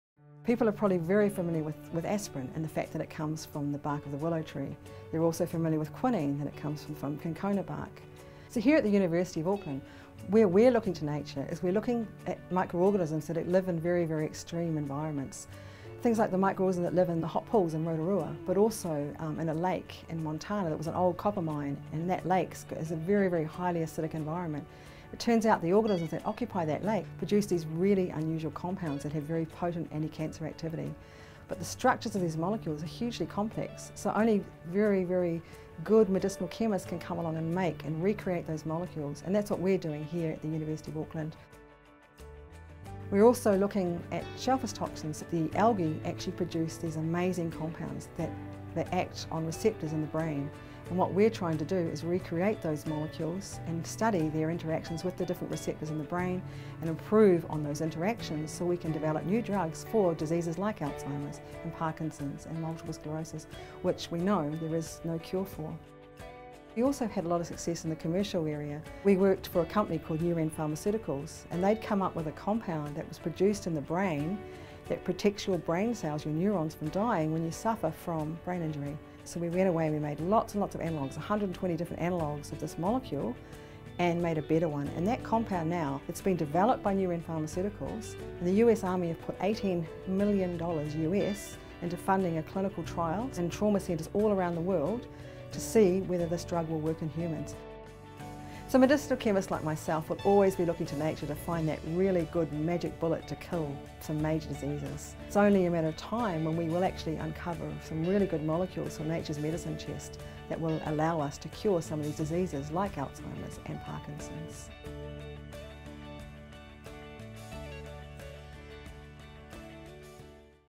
Does nature have a cure for Alzheimer's?
Ever wonder if treatments for "incurable" diseases can be found in nature's medicine chest?
Find out how University of Auckland researcher, Professor Margaret Brimble, is investigating the curative potential of molecules found in nature. With a treatment for brain injury trauma already in clinical trials, Professor Brimble and her team of researchers are developing drugs with the potential to cure diseases such as multiple sclerosis (MS), Alzheimer's and Parkinson's.